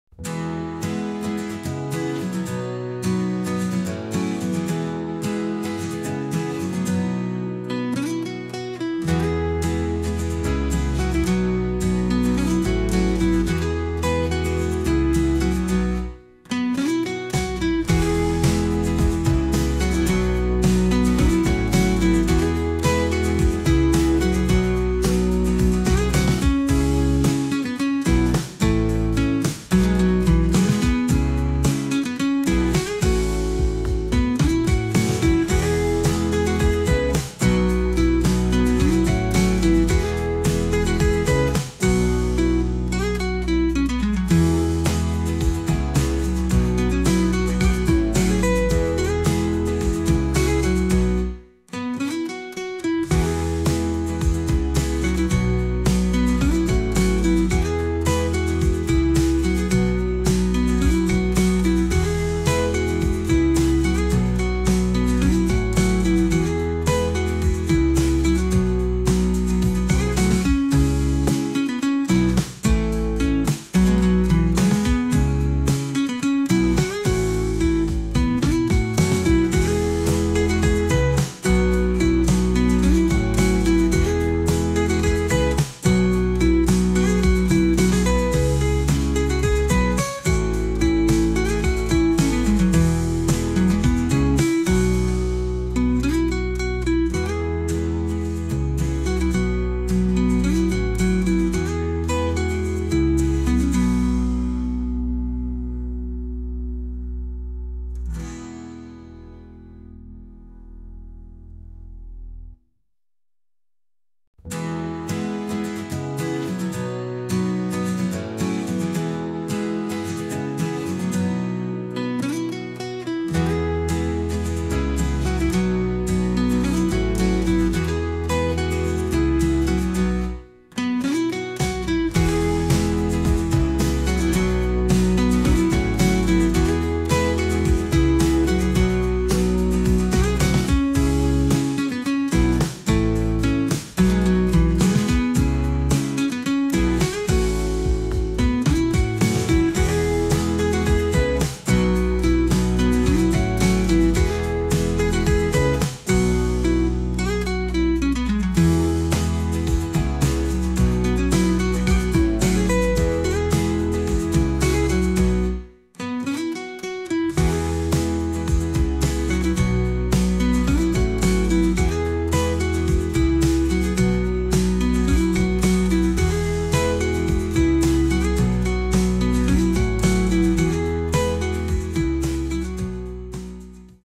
Klonování lidského hlasu pomocí AI
Jak vlastně funguje klonování lidského hlasu pomocí umělé inteligence?